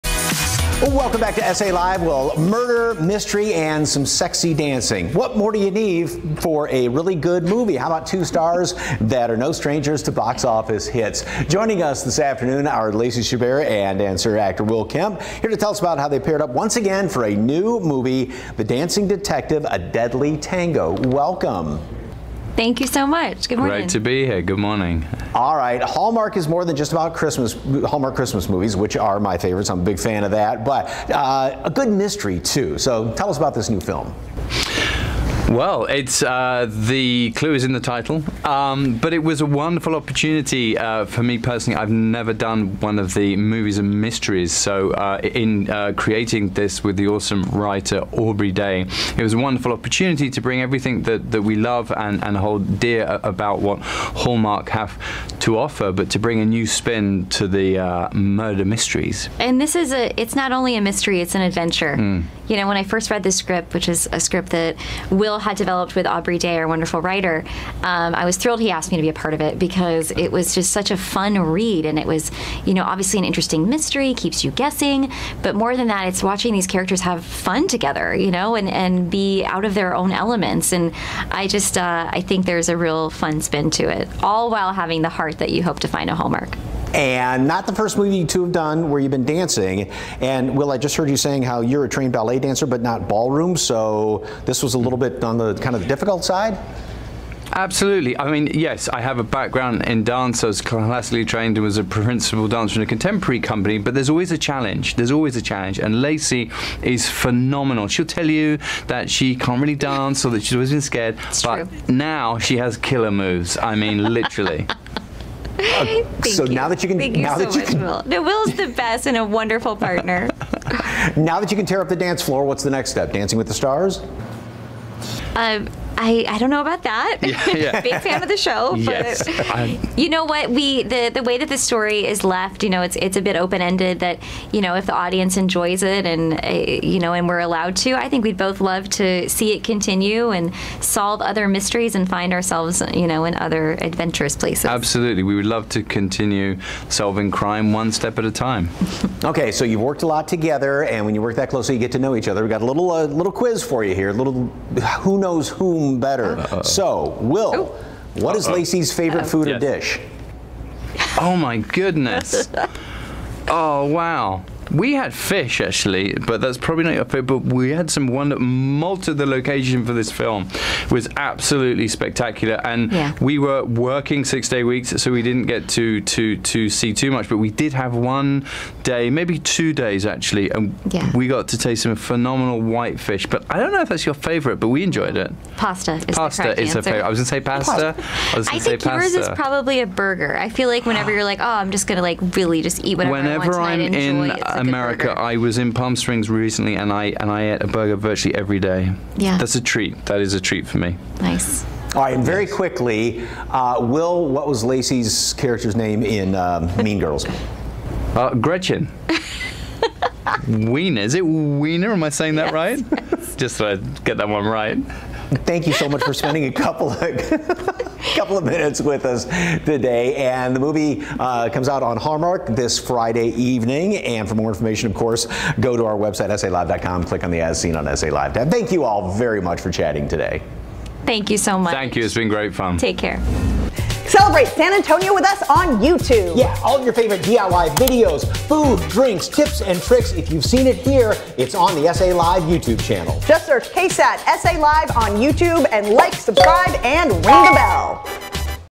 Celebrity Chat: Lacey Chabert dances her way into a new Hallmark movie premiering this weekend
Mean Girls star Lacey Chabert chats with us about her experience on The Dancing Detective: A Deadly Tango.